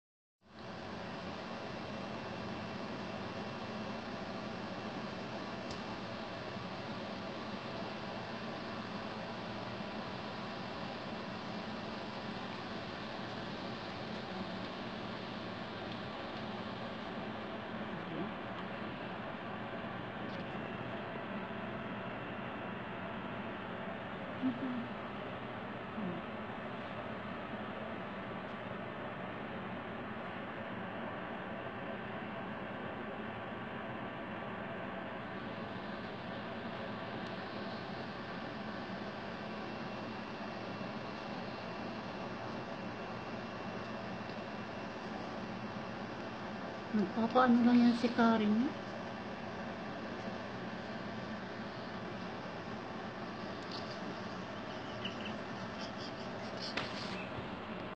OFWserye facialthreading...my MeTime..pampering myself
part 1 =facial threading with my friend Lizajane that was an old video...remembering the days...❤❤❤